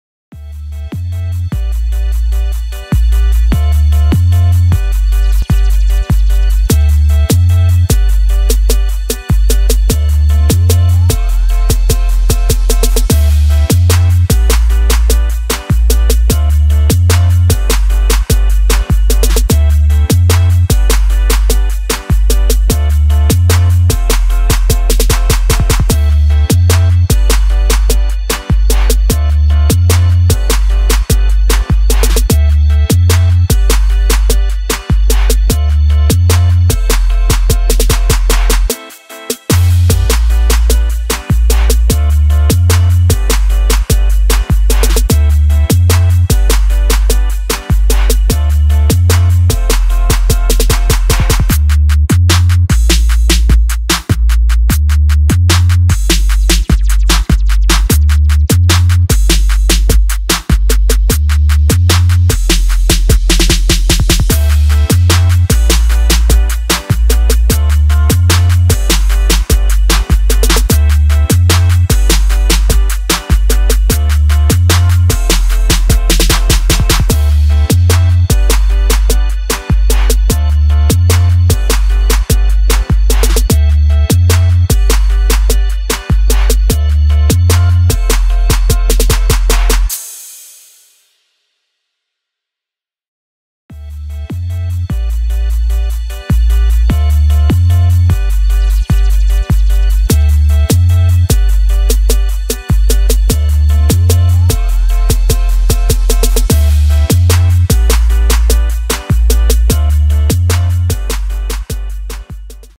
Hoverboard riding
Do tricks and race each other